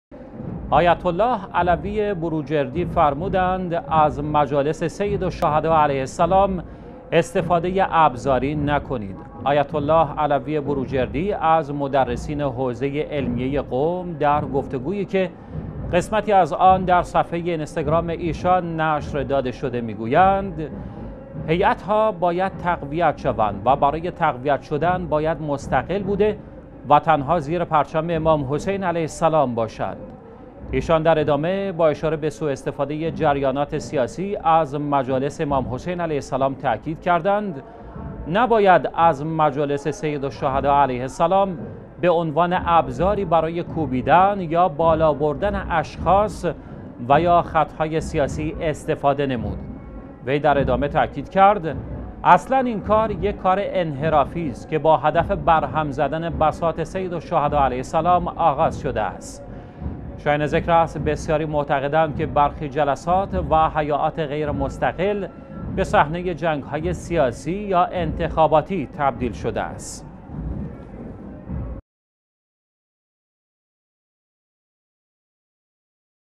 مجلس سید الشهدا علیه السلام جای مطالب سیاسی نیست
آیت الله علوی بروجردی فرمودند از مجالس سید الشهدا علیه السلام استفاده ابزاری نکنید.
آیت الله علوی بروجردی از مدرسین حوزه علمیه قم، در گفتگویی که قسمتی از آن در صفحه اینستاگرام ایشان نشر داده شده، می گویند: هیئت ها باید تقویت شوند و برای تقویت شدن باید مستقل بوده تنها زیر پرچم امام حسین علیه السلام باشند.
ایشان در ادامه با اشاره به سواستفاده جریانات سیاسی از مجالس امام حسین علیه السلام، تاکید کردند: نباید از مجالس سید الشهدا علیه السلام به عنوان ابزاری برای کوبیدن یا بالابردن اشخاص و یا خط های سیاسی استفاده نمود.
وی در ادامه تاکید کرد: اصلا این کار یک کار انحرافی است که با هدف برهم زدن بساط سید الشهدا علیه السلام آغاز شده است.
شایان ذکر است، بسیاری معتقدند که برخی جلسات و هیئات غیر مستقل به صحنه جنگ های سیاسی یا انتخاباتی تبدیل شده است.